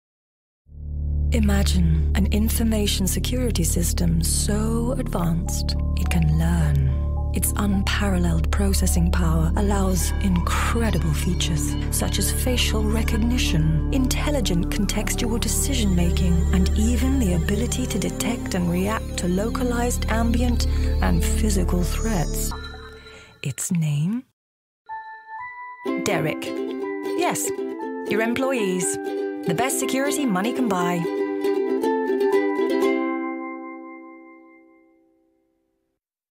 KnowBe4 | Security Awareness Training
KnowBe4's Enterprise Awareness Training Program provides you with a comprehensive new-school approach that integrates baseline testing using mock attacks, engaging interactive web-based training, and continuous assessment through simulated phishing and vishing attacks to build a more resilient and secure organization.